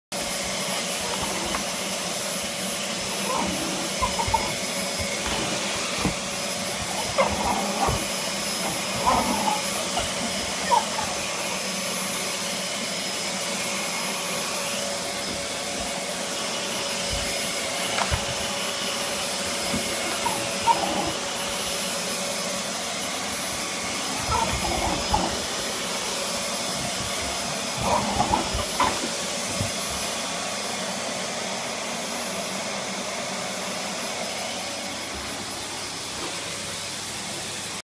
Miniature schnauzer VS vacuum cleaner
Chip the black miniature schnauzer Vs the vacuum cleaner